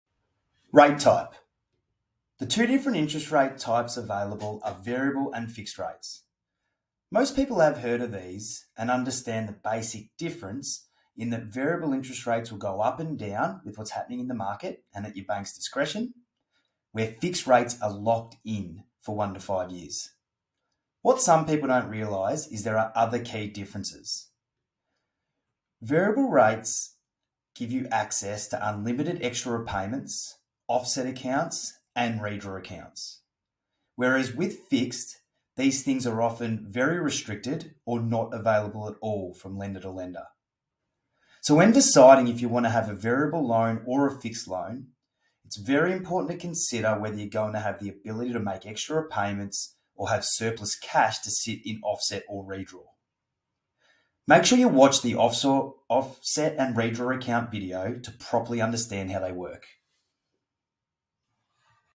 Home Loans - Variable vs Fixed rate types
Choosing between a fixed and variable home loan can be daunting. Here's a short overview from our head of lending, Sam Gawenda.